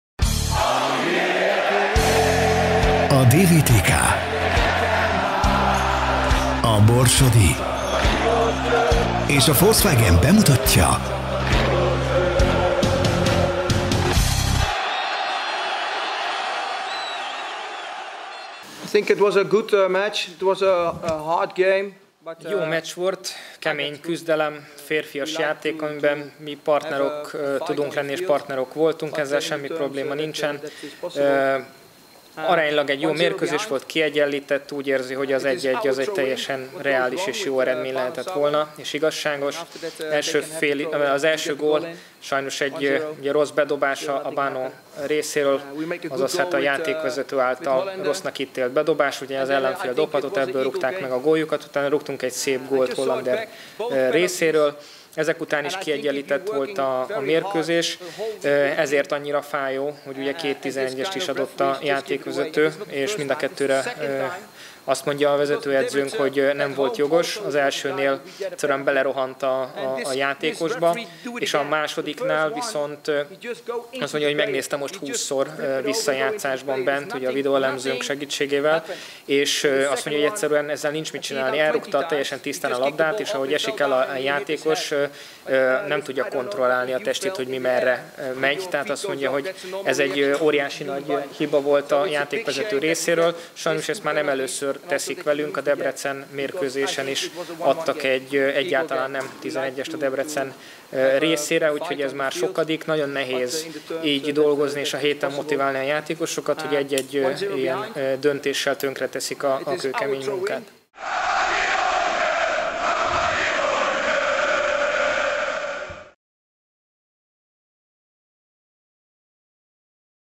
Erik van der Meer értékelése | DVTK - Honvéd | 2017. október 21. | DVTK TV
Diósgyőri VTK - Budapest Honvéd 2-1 (1-1)

Gólszerző: Ugrai (1-0) a 20., Holender (1-1) a 38., Ioannidis (2-1) a 90+4. percben.